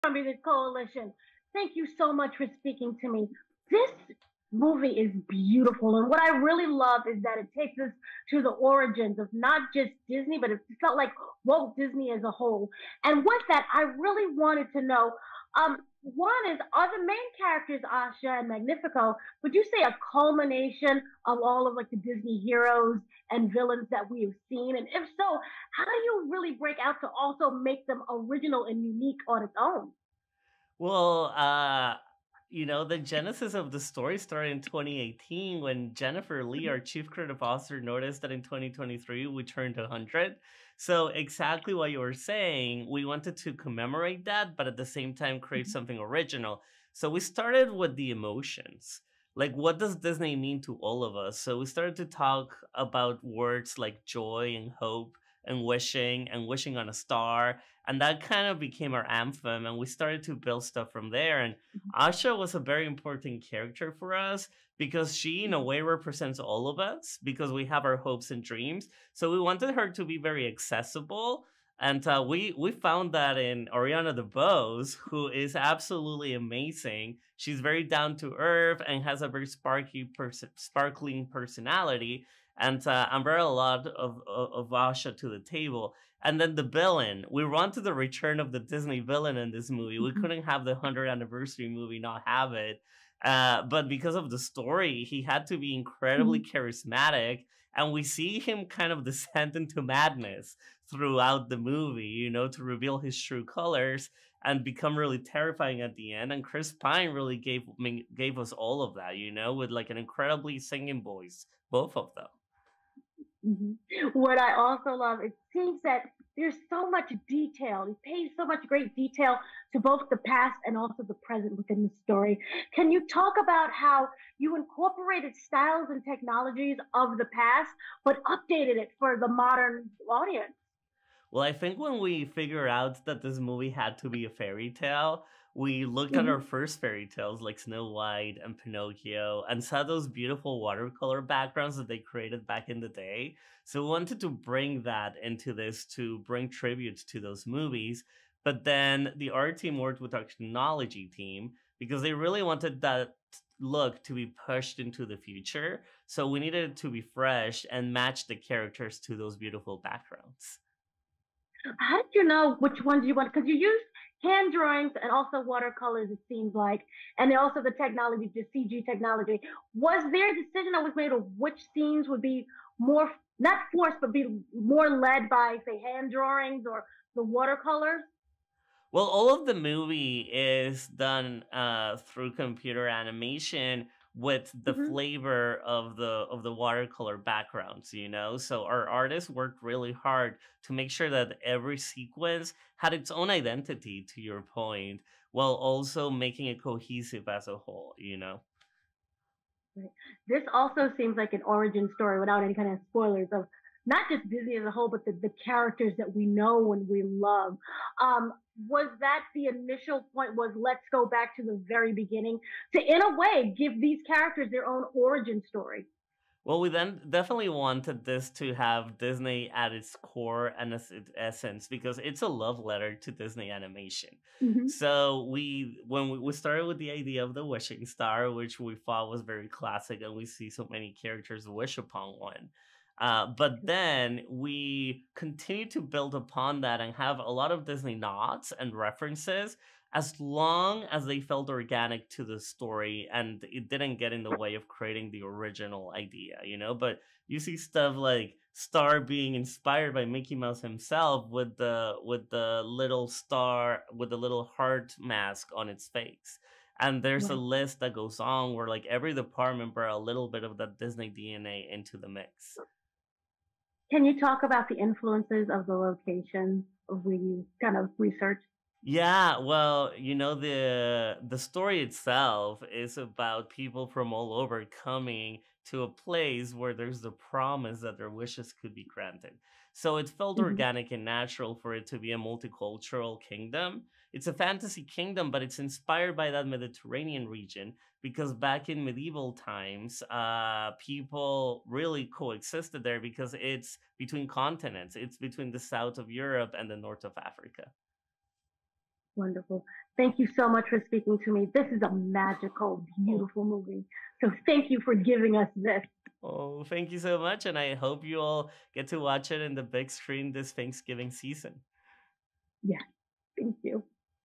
Juan Pablo Reyes Lancaster Jones Explains How Disney's Wish Pays Homage To The Past and Present
Our Entertainment Editor spoke to Disney's Wish production designer Juan Pablo Reyes Lancaster Jones to learn more about the creativity, inspiration and technology to bringing their movie to life and the history of Disney Animated Studios.

In Walt Disney Animation Studios’ all-new musical comedy “Wish,” audiences travel to the
magical kingdom of Rosas, where Asha, a sharp-witted idealist, makes a wish so powerful that it is answered by a cosmic force—a little ball of boundless energy called Star. Together, Asha and Star confront a most formidable foe—the ruler of Rosas, King Magnifico—to save her community and prove that when the will of one courageous human connects with the magic of the stars, wondrous things can happen.

The Koalition provides videos and podcasts relating to the latest news in both gaming and pop culture.